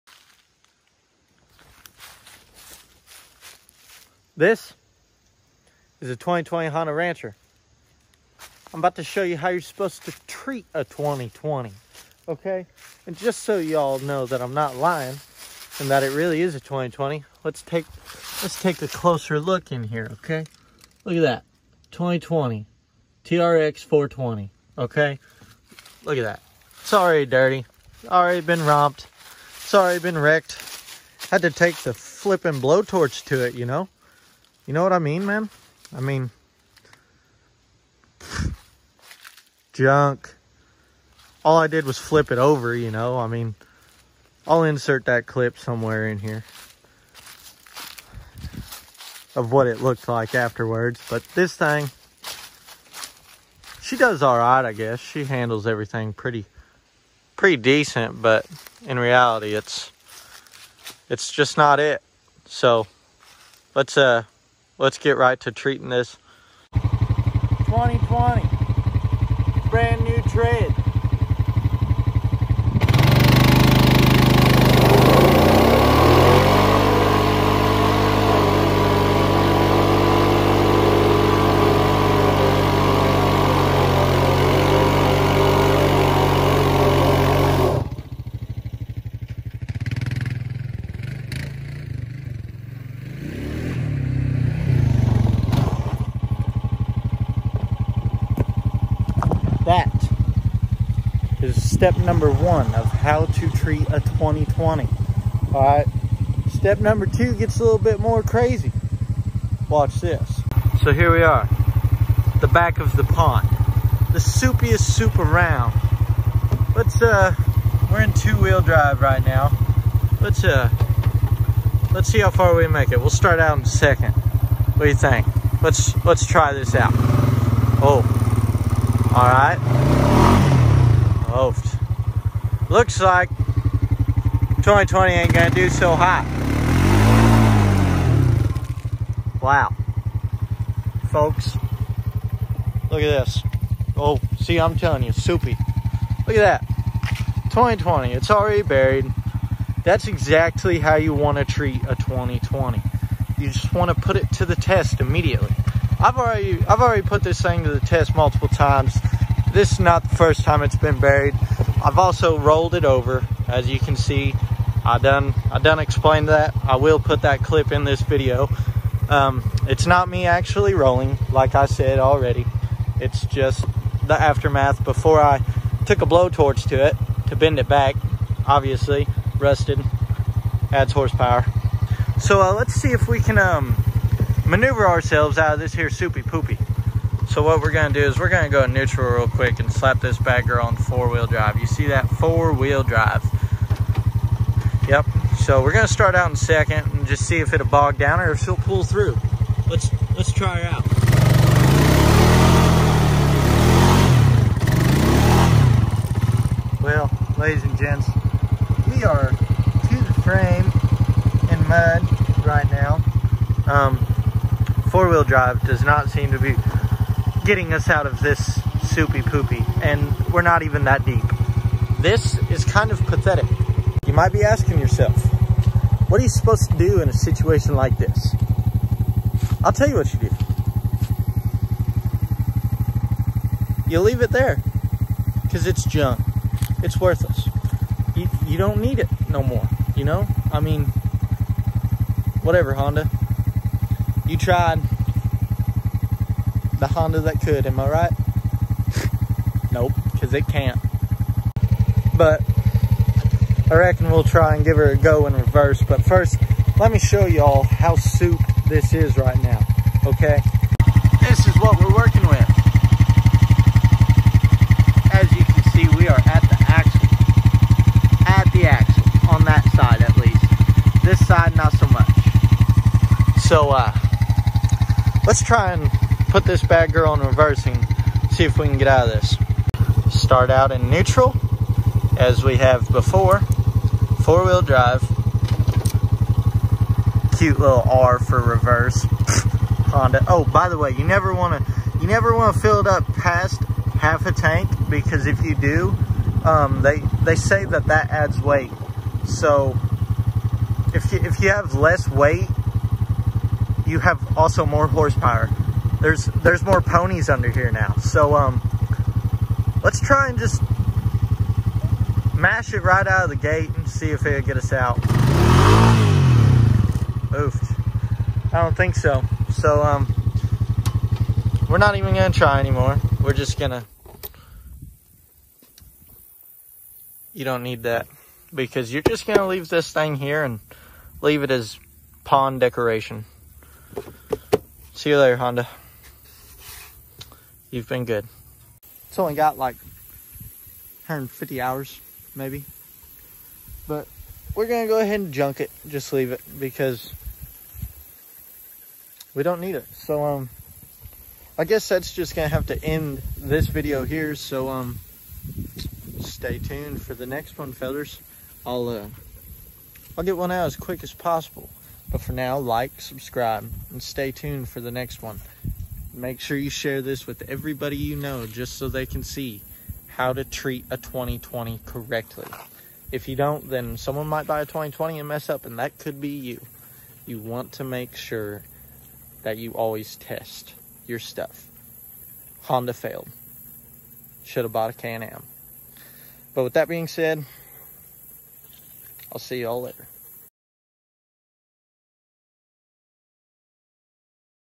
THE 2020 CAN’T HANG!!! JUNK HONDA GET’S BROKE IN!
This is strictly a teaser for the type of content that will be on this channel, there will also be normal daily vlogs! Daily videos are about to start, let me know your opinions in the comments!